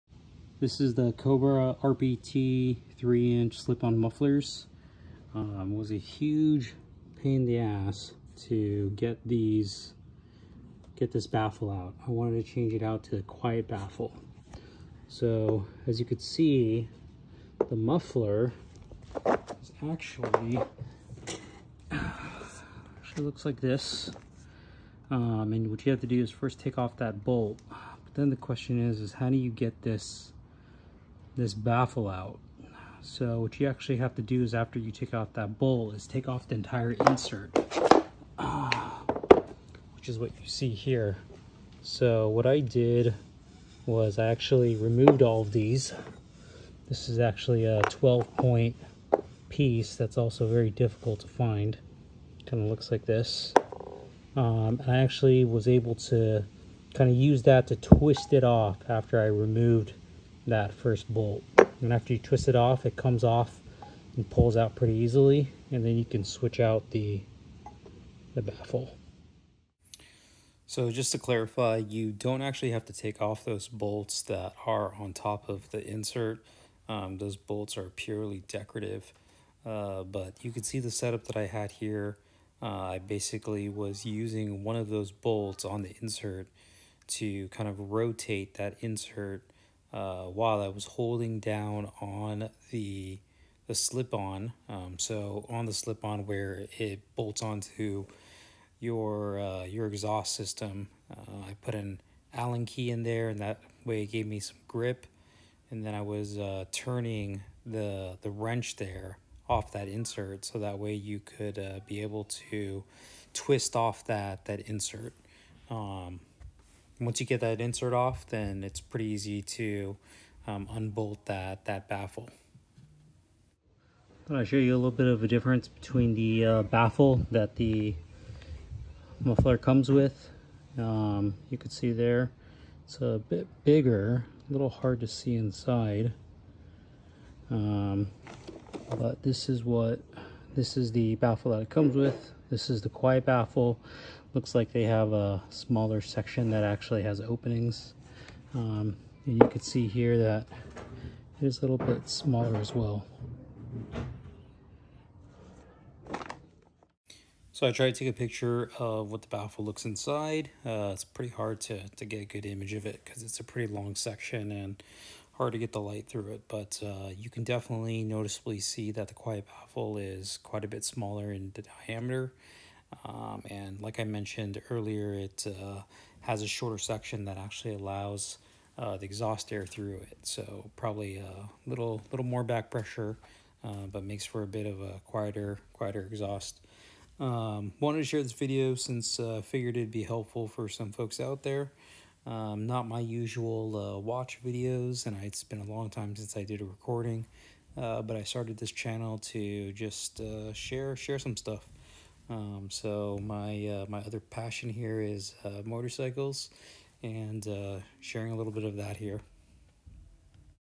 Cobra RPT Slip On Muffler - Harley Sportster | How to Remove Baffle and Quiet Baffle Comparison
It was such a pain figuring out how to remove the baffle, I figured it’d help some folks to share how I did it.

This is a how to on removing the baffle for the Cobra 3” RPT Slip On Muffler for my Harley Forty Eight